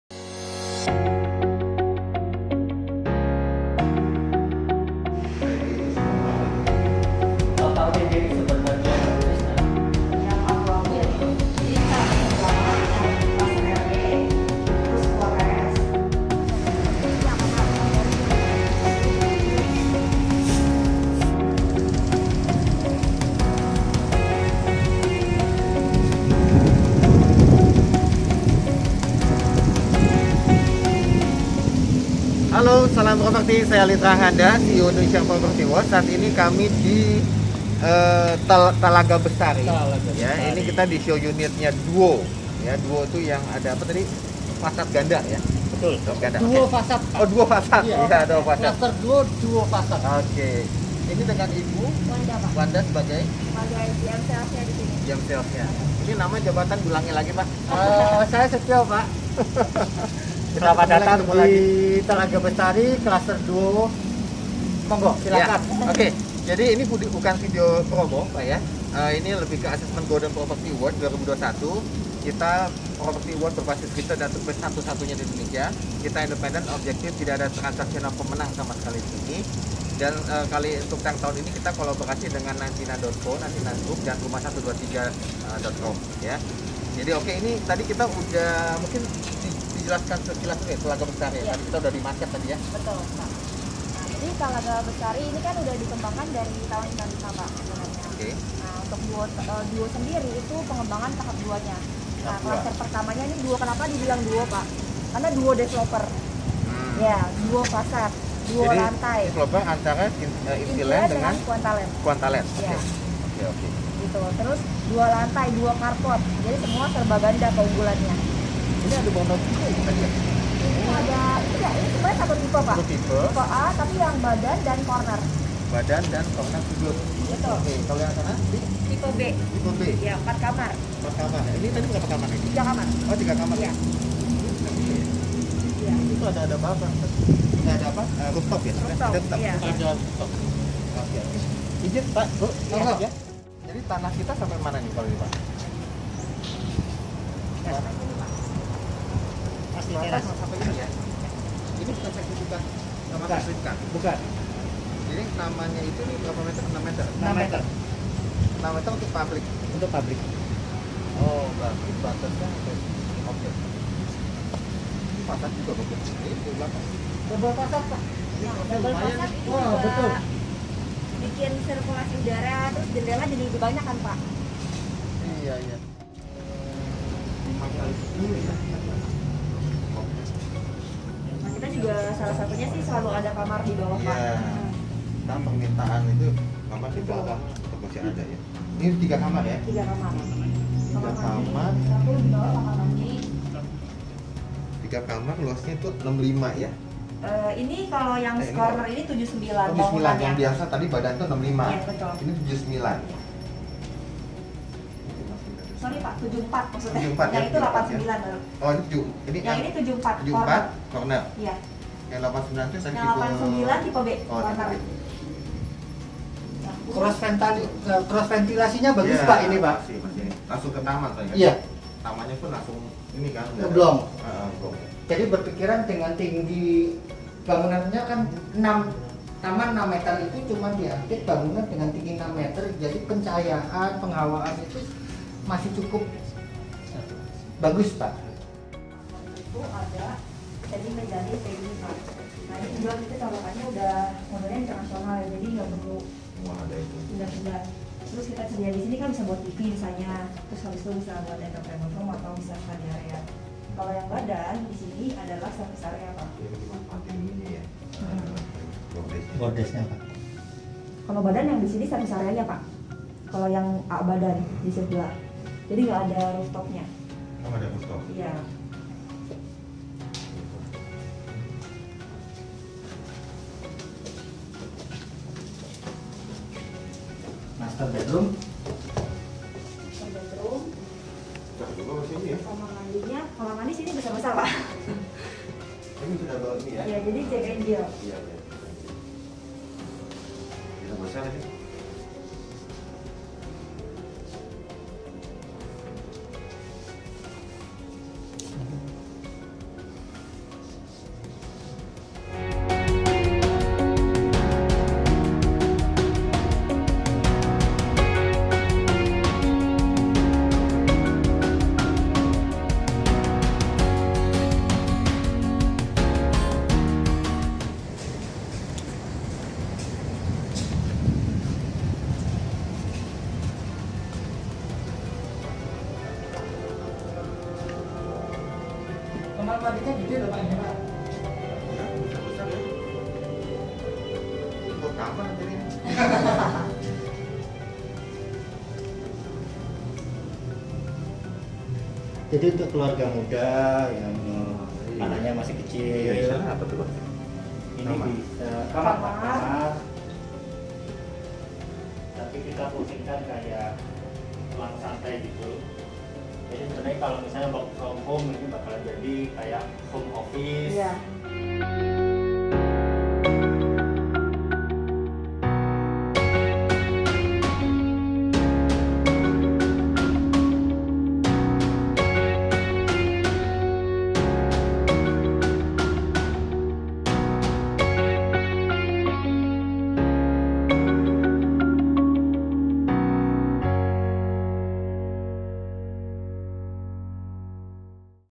Project Assessment GPA 2021 Duo @Talaga Bestari
DUO merupakan kawasan hunian yang dikembangkan oleh PT. Intiland Development, Tbk. dan PT. Quanta Land Indonesia dengan konsep Double All yakni terdapat 2 fasad, 2 Carpark, 2 taman dan 2 Club House.

Duo Fasad, Inovasi rumah dengan dua tampak yakni depan dan belakang, memanfaatkan ruangan dengan begitu maksimal. Juga didesain dengan jendela-jendela besar sehingga rumah mendapatkan pencahayaan alami.

Duo Carpark, dilengkapi dengan dua lahan parkir kendaraan, sehingga kenyamanan anda dan komunitas perumahaan selalu terjaga.

Duo Taman, Hunian DUO dilengkapi dengan dua taman di setiap rumah,